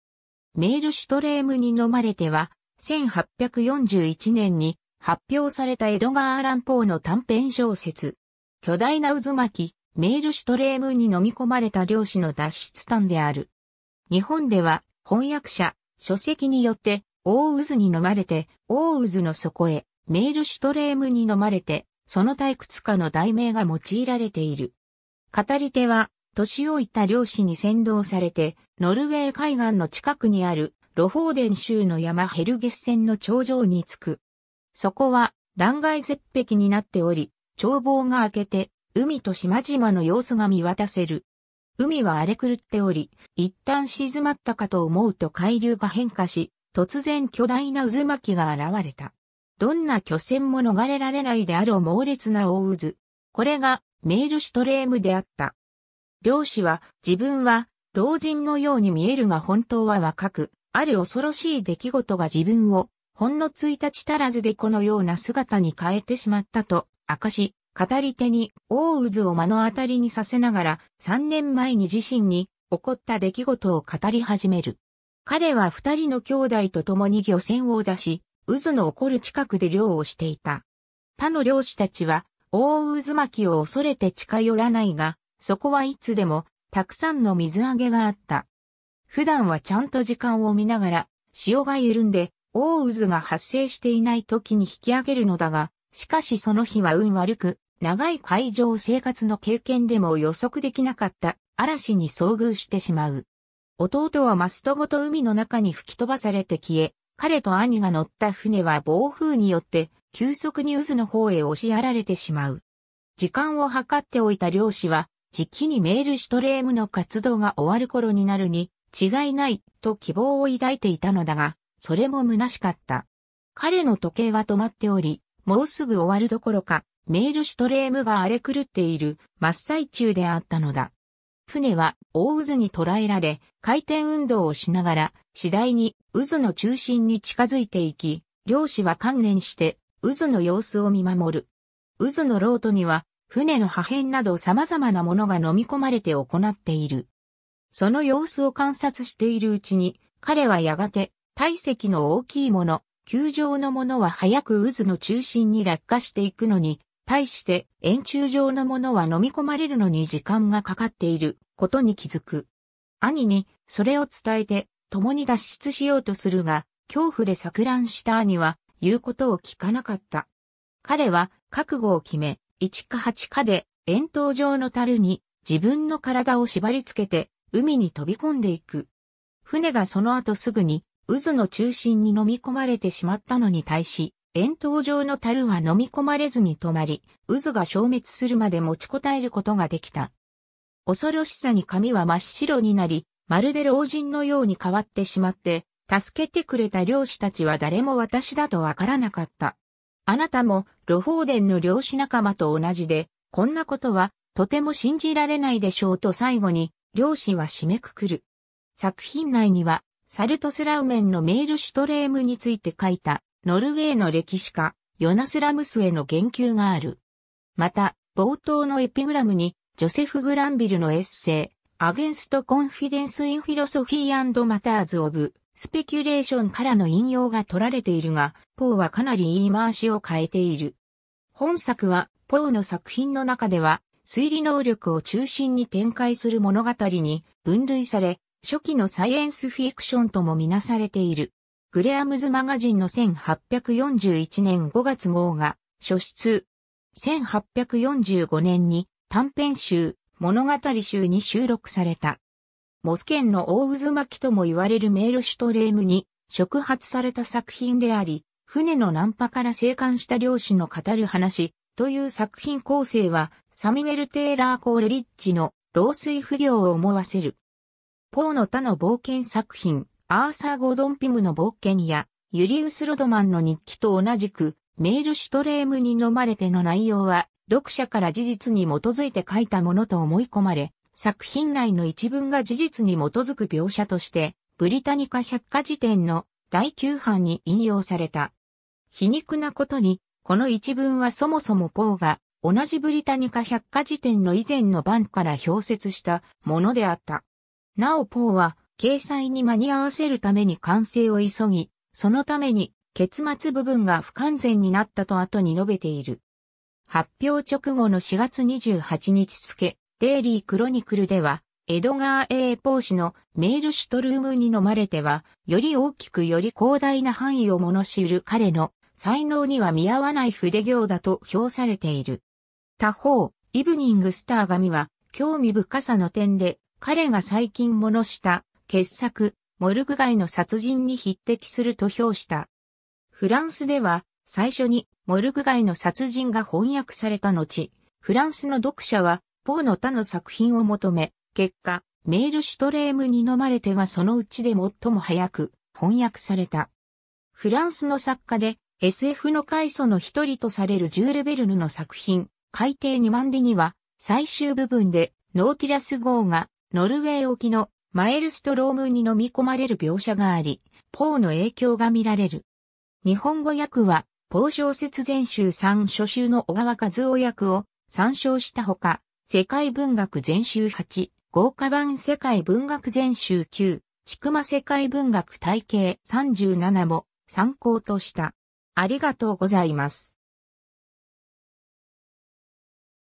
メエルシュトレエムに呑まれて
エドガー・アラン・ポーの短編小説
「メエルシュトレエムに呑まれて」（メエルシュトレエムにのまれて、A Descent into the Maelström）は、1841年に発表されたエドガー・アラン・ポーの短編小説。
巨大な渦巻「メエルシュトレエム」に呑み込まれた漁師の脱出譚である。
 日本では、翻訳者・書籍によって『大渦に呑まれて』、『大渦の底へ』、『メールシュトレームに呑まれて』その他幾つかの題名が用いられている。
 語り手は「年老いた」漁師に先導されて、ノルウェー海岸の近くにあるロフォーデン州の山ヘルゲッセンの頂上に着く。
そこは断崖絶壁になっており、眺望が開けて海と島々の様子が見渡せる。
海は荒れ狂っており、一旦静まったかと思うと海流が変化し、突然巨大な渦巻きが現れた。
どんな巨船も逃れられないであろう猛烈な大渦。
これが「メエルシュトレエム」であった。
漁師は、自分は老人のように見えるが本当は若く、ある恐ろしい出来事が自分をほんの1日足らずでこのような姿に変えてしまったと明かし、語り手に大渦を目の当たりにさせながら、3年前に自身に起こった出来事を語り始める。
 彼は二人の兄弟とともに漁船を出し、渦の起こる近くで漁をしていた。
他の漁師たちは大渦巻きを恐れて近寄らないが、そこはいつでもたくさんの水揚げがあった。
普段はちゃんと時間を見ながら、潮が緩んで大渦が発生していない時に引き上げるのだが、しかしその日は運悪く、長い海上生活の経験でも予測できなかった嵐に遭遇してしまう。
弟はマストごと海の中に吹き飛ばされて消え、彼と兄が乗った船は暴風によって急速に渦の方へ押しやられてしまう。
時間を計っておいた漁師は、じきにメエルシュトレエムの活動が終わる頃になるに違いない、と希望を抱いていたのだが、それも空しかった。
彼の時計は止まっており、もうすぐ終わるどころか、メエルシュトレエムが荒れ狂っている真っ最中であったのだ。
 船は大渦に捉えられ、回転運動をしながら次第に渦の中心に近づいていき、漁師は観念して渦の様子を見守る。
渦の漏斗には船の破片など様々なものが飲み込まれて行っている。
その様子を観察しているうちに、彼はやがて、体積の大きいもの、球状のものは早く渦の中心に落下して行くのに対して、円柱状のものは飲み込まれるのに時間がかかっていることに気付く。
兄にそれを伝えて共に脱出しようとするが、恐怖で錯乱した兄は言う事を聞かなかった。
彼は覚悟を決め、一か八かで円筒状の樽に自分の体を縛り付けて海に飛び込んでいく。
船がそのあとすぐに渦の中心に飲み込まれてしまったのに対し、円筒状の樽は飲み込まれずに留まり、渦が消滅するまで持ちこたえることができた。
「恐ろしさに髪は真っ白になり、まるで老人のように変わってしまって、助けてくれた漁師たちは誰も私だとわからなかった。あなた（語り手）もロフォーデンの漁師仲間と同じで、こんな事はとても信じられないでしょう」と最後に漁師は締めくくる。
 作品内にはサルトスラウメンのメエルシュトレエムについて書いたノルウェーの歴史家ヨナス・ラムスへの言及がある。
また冒頭のエピグラムにジョセフ・グランヴィルのエッセイ『Against Confidence in Philosophy and Matters of Speculation』（1676年）からの引用が取られているが、ポーはかなり言い回しを変えている。
 本作は、ポーの作品の中では「推理能力を中心に展開する物語（tales of ratiocination）」に分類され、初期のサイエンス・フィクション（SF）とも見なされている。
『グレアムズ・マガジン』の1841年5月号が初出。
1845年に短編集『物語集』に収録された。
 「モスケンの大渦巻き」とも言われるメエルシュトレエムに触発された作品であり、船の難破から生還した漁師の語る話、という作品構成はサミュエル・テイラー・コールリッジの『老水夫行』（The Rime of the Ancient Mariner、1798年）を思わせる。
ポーの他の冒険作品『アーサー・ゴードン・ピムの冒険』や『ユリウス・ロドマンの日記』と同じく、「メエルシュトレエムに呑まれて」の内容は読者から事実に基づいて書いたものと思い込まれ、作品内の一文が事実に基づく描写としてブリタニカ百科事典の第9版に引用された。
皮肉なことに、この...